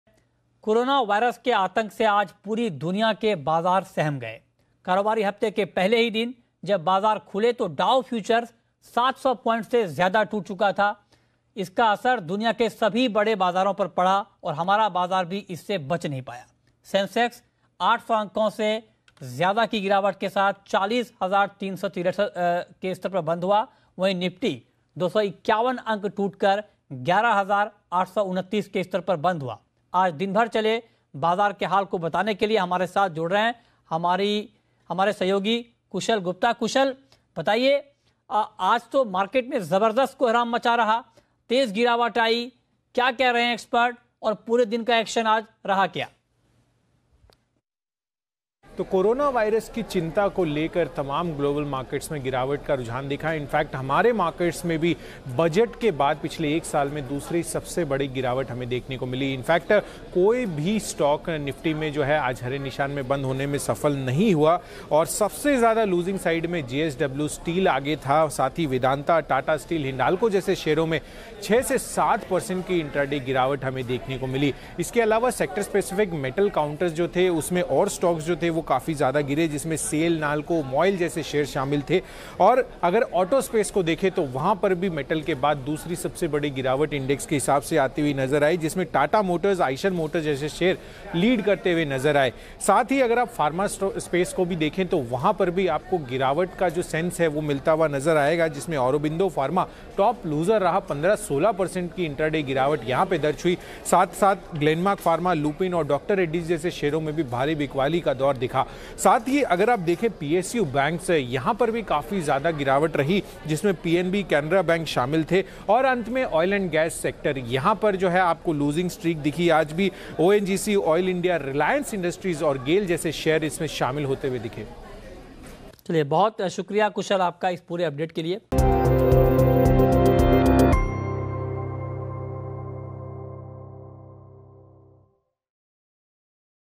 Sensex crashes over 800 points, Nifty ends below 11,850
The Sensex tanked over 807 points to 40,363 while the broader Nifty50 index slid below 11,850 to settle at 11,838.

Zee Business: shorturl.at/hpqX6
WION: shorturl.at/fwKO0
Zee News English: shorturl.at/aJVY3
Zee News Hindi: shorturl.at/eorM1
DNA News: shorturl.at/rBOY6
BGR: shorturl.at/eioqL